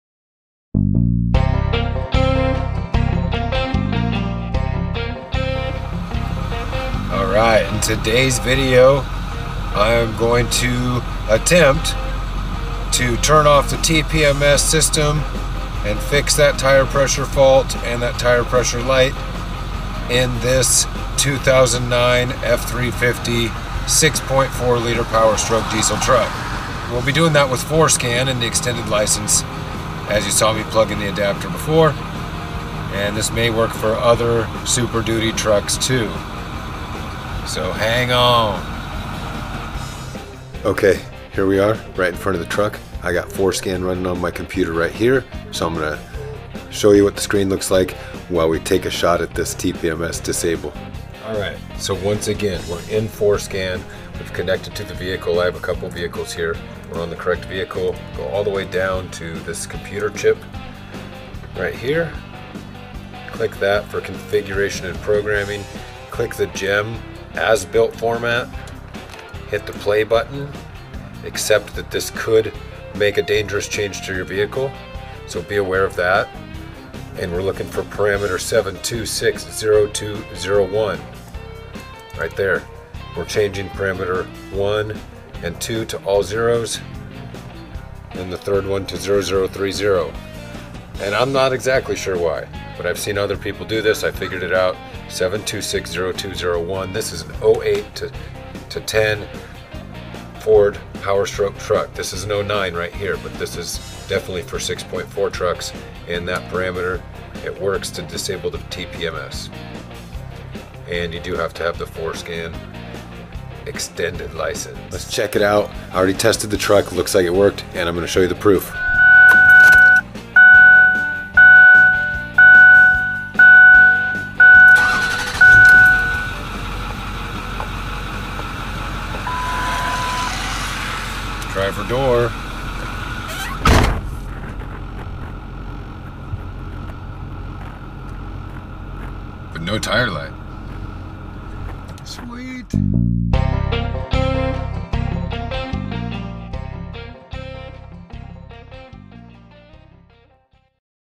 Forscan F350 TPMS Disable - Ford Super Duty F250/F350 2008-2010
In this video you'll find out how to disable TPMS in a Ford Super Duty truck like an F350 using Forscan. You can use Forscan to disable TPMS on an F250 F350 and higher trucks. This particular video is for Forscan on a 2009 F350 Tire Pressure Monitoring System fault code. I use Forscan to turn off the tire light in my F350.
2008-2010 Ford Super Duty 6.4 liter diesel truck. How to disable TPMS in an F350?